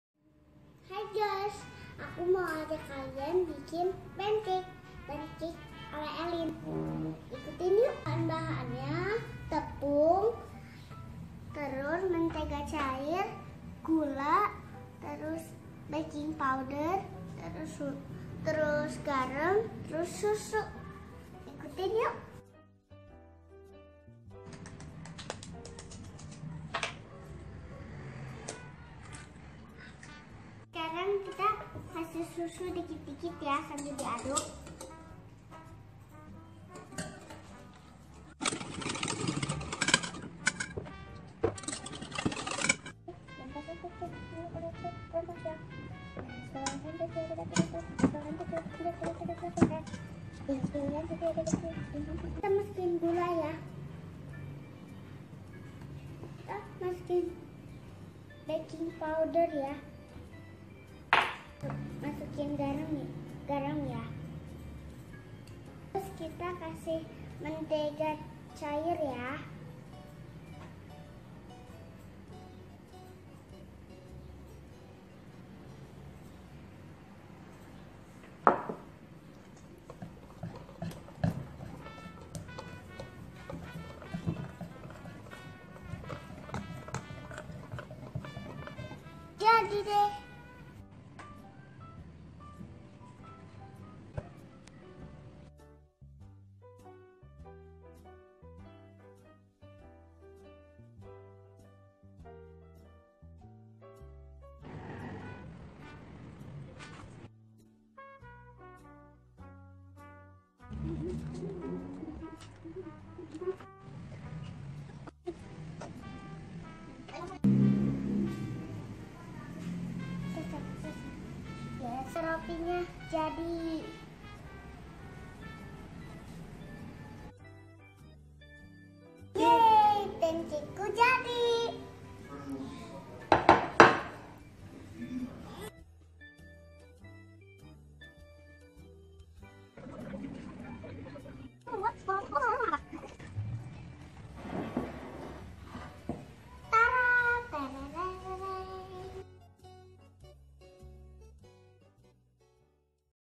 PANCAKE KARAKTER ALA ELLYN
Pancake karakter 😍😍😍 ... membuat pancake bersama keluarga di rumah adalah salah satu ide mengisi liburan nih 😉... dimana bisa melatih kreatifitas😎 dan kehangatan keluarga 🤗 n yg pasti ningkatin selera makan anak 😋 ... btw mau tau apa itu pancake  kan ? ... pancake ini adalah pilihan menu sarapan yang mudah dibuat dan disukai anak anak 😍😍😍... apalagi dengan bentuk dan warna yg menarik sesuai keinginan anak anak ... yukk intip resepnya dan coba deh hasilnya 😍😘... yg pasti yummy ... 😍😍😍